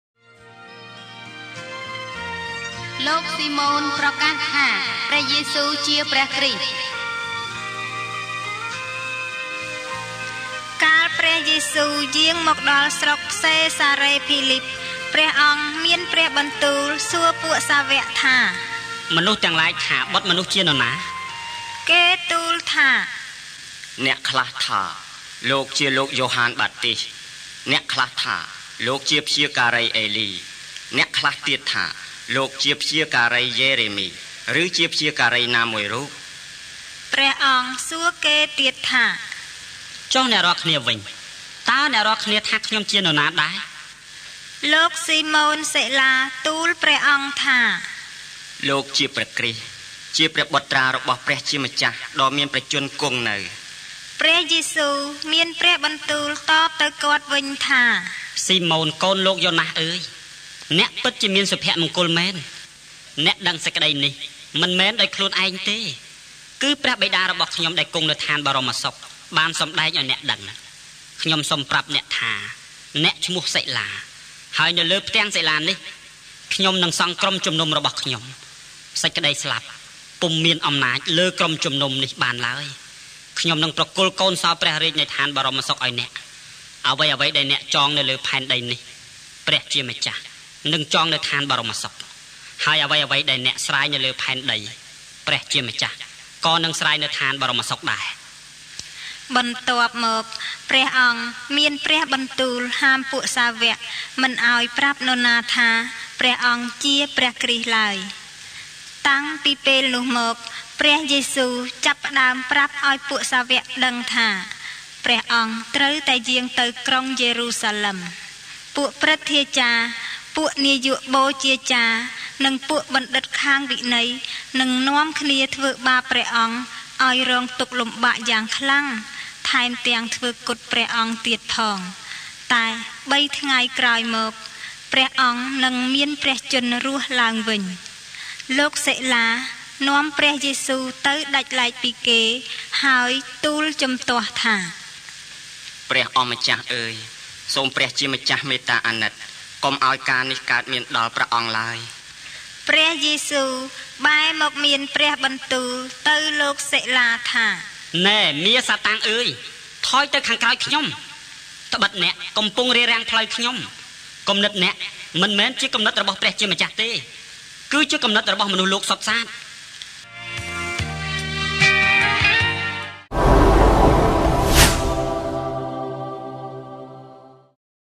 អត្ថបទគម្ពីរដំណឹងល្អ ម៉ាថាយ ១៦,១៣ - ២៣
លោក​ស៊ីម៉ូន​ប្រកាស​ថា ព្រះយេស៊ូ​ជា​ព្រះគ្រីស្ដ
(ម៉ាកុស ៨:២៧-៣០ លូកា ៩:១៨-២១)
១៣ កាល​ព្រះយេស៊ូ​យាង​មក​ដល់​ស្រុក​សេសារេភីលីព ព្រះអង្គ​មាន​ព្រះបន្ទូល​សួរ​ក្រុមសាវ័ក​​ថា៖ «មនុស្ស​ទាំងឡាយ​ថា​បុត្រ​មនុស្ស​ជា​នរណា?»។ ១៤គេ​ទូល​ថា៖ «អ្នក​ខ្លះ​ថា​លោក​ជា​លោក​យ៉ូហានបាទីស្ដ អ្នក​ខ្លះ​ថា​លោក​ជា​ព្យាការី​អេលី អ្នក​ខ្លះ​ទៀត​ថា លោក​ជា​ព្យាការី​យេរេមី ឬ​ជា​ព្យាការី​ណា​មួយ​រូប»។ ១៥​ ព្រះ​អង្គ​សួរ​គេ​ទៀត​ថា៖ «ចុះ​អ្នក​រាល់​គ្នា​វិញ តើ​អ្នក​រាល់​គ្នា​ថា ខ្ញុំ​ជា​នរណា​ដែរ ? »។ ១៦លោក​ស៊ីម៉ូនសិលា​ទូល​ព្រះ​អង្គ​ថា៖ «លោក​ជា​ព្រះគ្រីស្ដ ជា​ព្រះបុត្រា​របស់​ព្រះជា​ម្ចាស់​​​​​​​ដ៏​មាន​ព្រះជន្ម​គង់​នៅ»។ ១៧ ព្រះយេស៊ូ​មាន​ព្រះបន្ទូល​តប​ទៅ​គាត់​វិញ​ថា៖ «ស៊ីម៉ូន​កូន​លោក​យ៉ូណាស​អើយ អ្នក​ពិត​ជា​មាន​សុភមង្គល​មែន អ្នក​ដឹង​សេចក្ដី​នេះ មិន​មែន​ដោយ​ខ្លួនឯង​ទេ គឺ​ព្រះបិតា​របស់​ខ្ញុំ​ដែល​គង់​នៅ​ស្ថាន​បរមសុខ​បាន​សំដែង​អោយ​អ្នក​ដឹង។ ១៨ ខ្ញុំ​សុំ​ប្រាប់​អ្នក​ ថា​អ្នក​ឈ្មោះ​សិលា ហើយ​នៅ​លើ​ផ្ទាំង​សិលា​នេះ ខ្ញុំ​នឹង​សង់​ព្រះសហគមន៍​របស់​ខ្ញុំ។ សេចក្ដីស្លាប់ពុំ​មាន​អំណាច​លើ​ព្រះសហគមន៍​នេះ​បាន​ឡើយ ១៩ ខ្ញុំ​នឹង​ប្រគល់​កូន​សោ​ព្រះរាជ្យ​នៃ​ស្ថាន​បរមសុខ​អោយ​អ្នក។ អ្វីៗ​ដែល​អ្នក​ចង​នៅ​លើ​ផែនដី​នេះ ព្រះជាម្ចាស់​នឹង​ចង​នៅ​ស្ថាន​បរមសុខ ហើយ​អ្វីៗ​ដែល​អ្នក​ស្រាយនៅ​លើ​ផែនដី ព្រះជាម្ចាស់​ក៏​នឹង​ស្រាយ​នៅ​ស្ថាន​បរមសុខ​ដែរ»។ ២០ បន្ទាប់​មក ព្រះអង្គ​មាន​ព្រះបន្ទូល​ហាម​ក្រុមសាវ័ក​​មិន​អោយ​​​​​​ប្រាប់​នរណា​ថា ព្រះអង្គ​ជា​ព្រះគ្រីស្ដ​ឡើយ។

២១ តាំង​ពី​ពេល​នោះ​មក ព្រះយេស៊ូ​​ចាប់​ផ្ដើម​ប្រាប់​អោយ​ក្រុមសាវ័ក​ដឹង​ថា ព្រះអង្គ​ត្រូវ​តែ​យាង​ទៅ​ក្រុង​យេរូសាឡឹម។ ពួក​ព្រឹទ្ធាចារ្យ* ពួក​នាយក​បូជាចារ្យ* និង​ពួកធម្មចារ្យ​នឹង​នាំ​គ្នា​ធ្វើ​បាប​ព្រះអង្គ​អោយ​រង​ទុក្ខ​លំបាក​យ៉ាង​ខ្លាំង ថែម​ទាំង​ធ្វើ​គុត​ព្រះអង្គ​ទៀត​ផង​តែ​បី​ថ្ងៃ​ក្រោយ​មក ព្រះអង្គ​នឹង​មាន​ព្រះ​ជន្ម​រស់​ឡើង​វិញ។

២២ លោក​សិលា​នាំ​ព្រះយេស៊ូ​ទៅ​ដាច់​ឡែក​ពី​គេ ហើយ​ទូល​ជំទាស់​ថា៖ «ព្រះអម្ចាស់​អើយ ! សូម​ព្រះជាម្ចាស់​មេ​ត្តា​អាណិត កុំ​អោយ​ការណ៍​នេះកើត​មាន​ដល់​ព្រះអង្គ​ឡើយ»។ ២៣ ព្រះយេស៊ូ​បែរ​មក​មាន​ ព្រះបន្ទូល​ទៅ​លោក​សិលា​ថា៖ «នែ៎ មារ​សាតាំង​អើយ ! ថយ​ទៅ​ខាង​ក្រោយ​ខ្ញុំ ដ្បិត​អ្នក​កំពុង​រារាំង​ផ្លូវ​ខ្ញុំ គំនិត​អ្នក​មិន​មែន​ជា​គំនិត​របស់​ព្រះជាម្ចាស់​ទេ គឺ​ជា​គំនិត​របស់​មនុស្ស​លោក​សុទ្ធសាធ»។